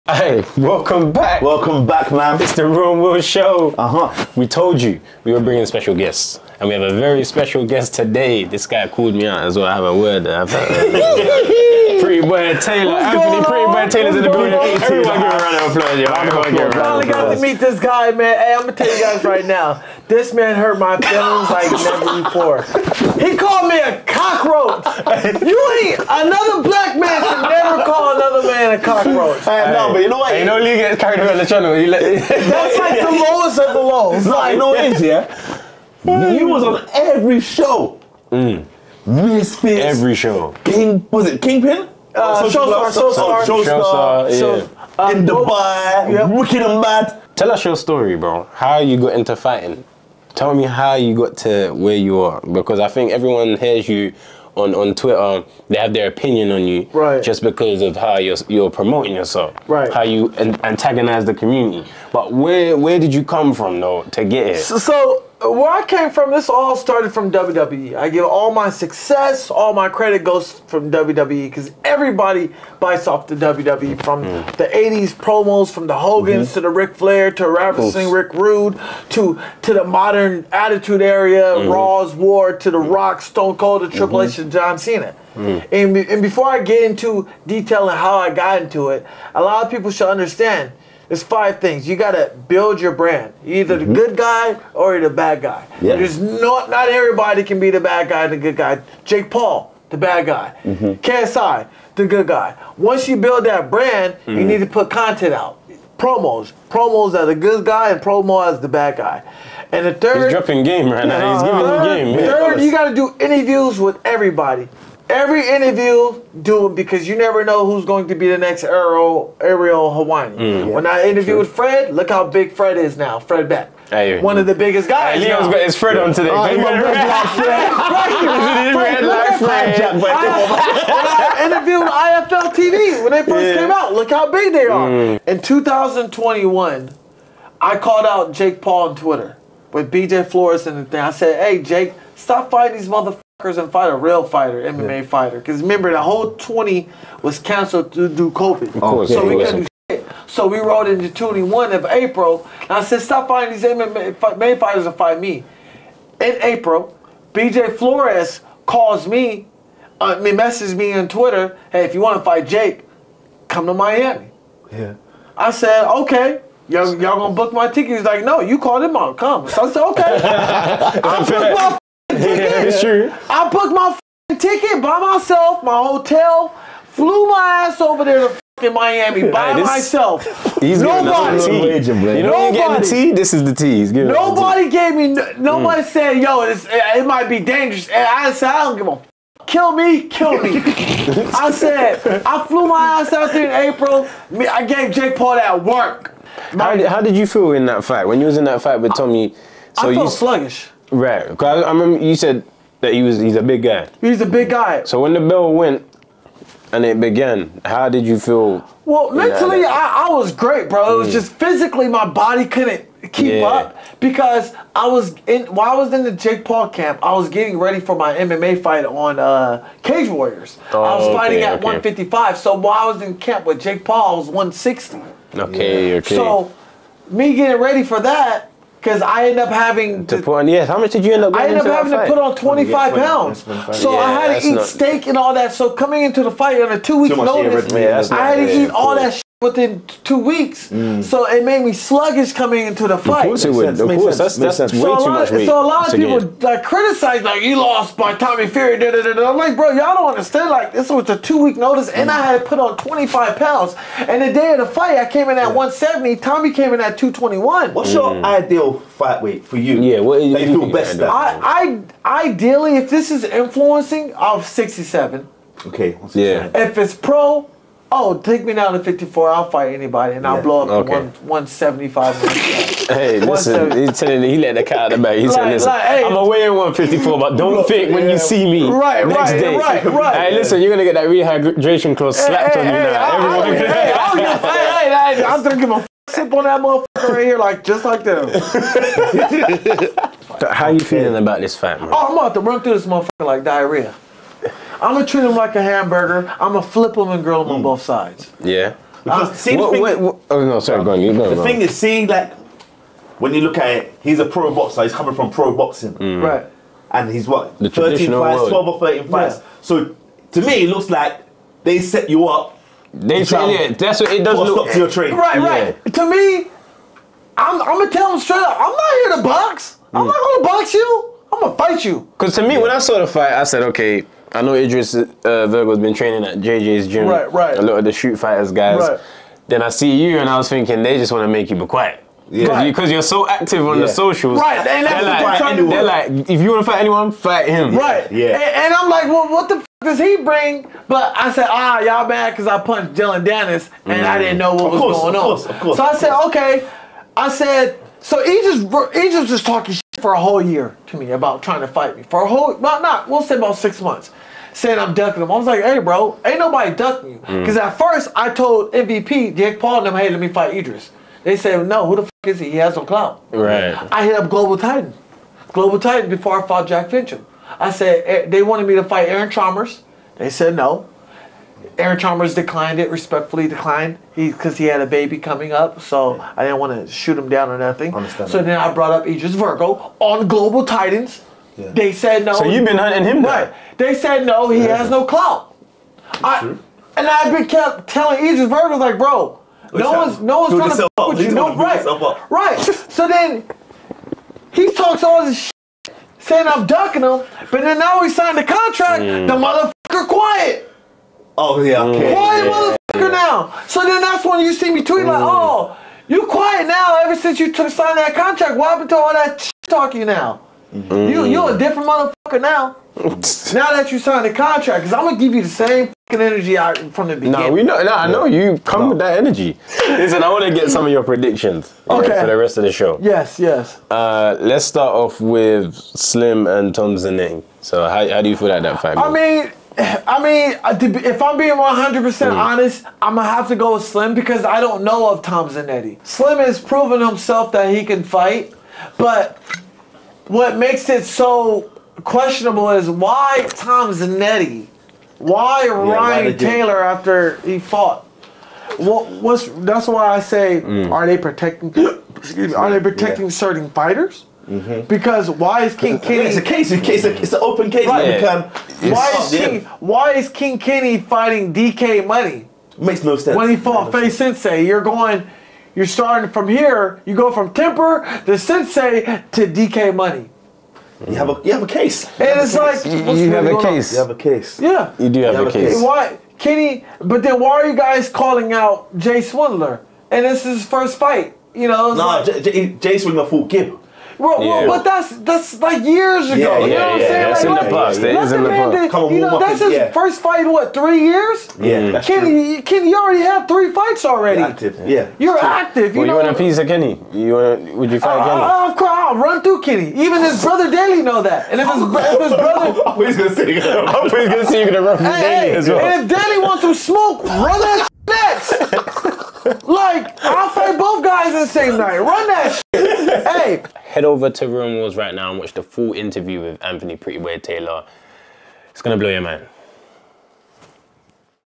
Viddal Riley Finally Confronts Anthony Taylor On Calling Him Out…
After Anthony Taylor calls out Viddal on Twitter, this was a MUST for the start of Fight Week Misfits Series 004 - FULL

MyProtein - ''VIDDAL" on checkout for discounts.

Capture your gameplay the same way THE RILIST Does!